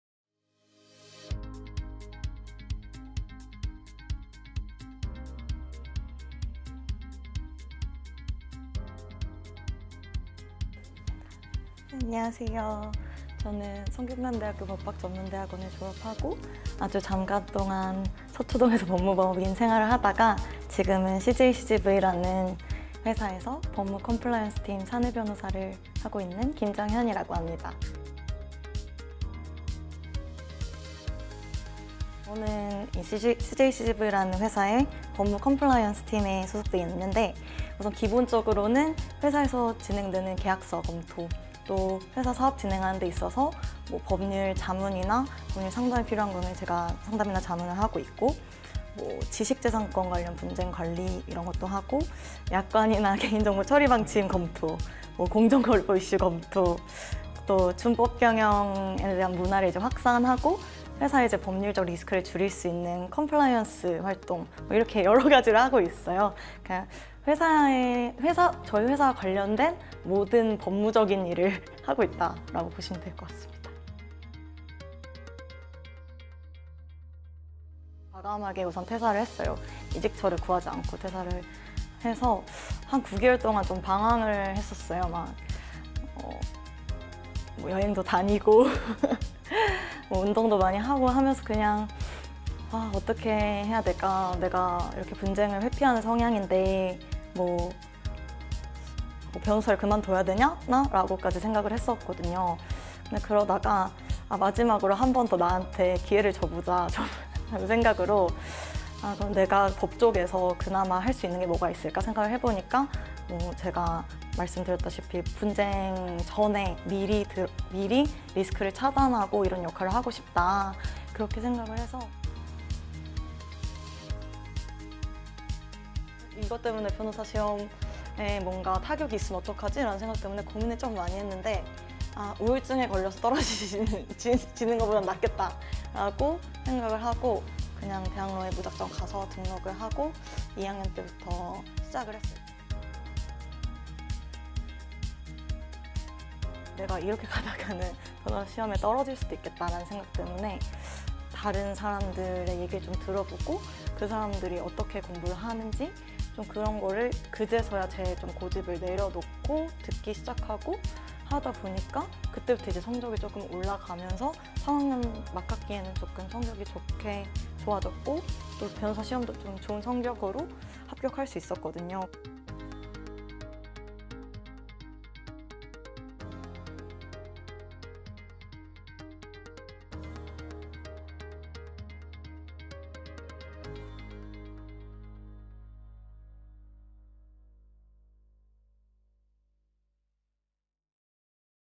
제6회 알쓸학잡 맛보기｜분쟁이 싫은 변호사
출연: 김정현 동문(CJ CGV 법무·컴플라이언스팀 변호사)
풀영상: 제6회 알쓸학잡 – 대학원생은 어떻게 취뽀할까?

우리학교 대학원 졸업 후 취뽀에 성공하신 선배님께 듣는 인생 경험담과 꿀팁 !
학자의 길을 가는 당신을 위한 생생한 사이다 토크 !
알아두면 쓸데'있'는 학자의 길에 대한 잡학사전, 알쓸학잡 !